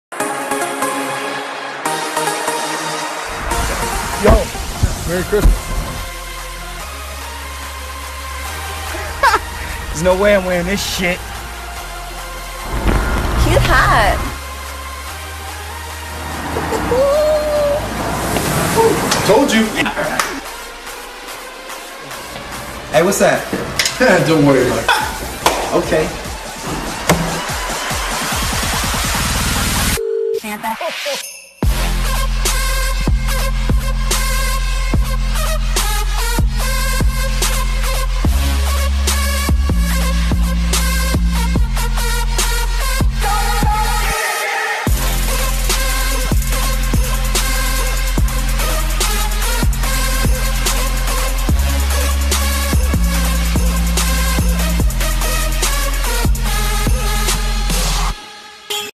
Sam Asghari @samasghari as Santa is HOT HOT HOT
Sam Asghari @samasghari Just made the hottest Christmas video. Can we get down with Santa and Rudolph (KING SAM JONES III )? OMG this video is hot hot hot!!!

We're talking EPIC ménage à trois Santa, Rudolph and you playing with their Christmas toys ...This is NOT for kids!